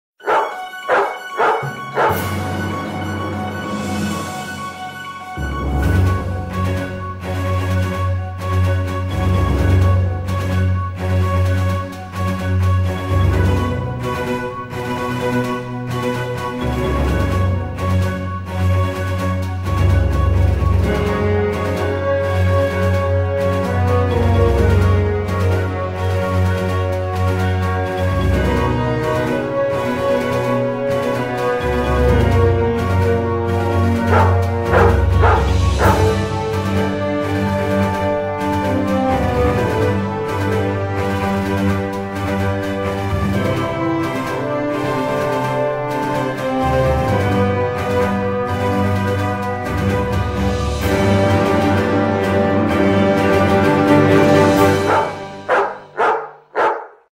Wiener Dog VS Laser!
This is the family's new puppy, Sam. HAPPY NEW YEAR!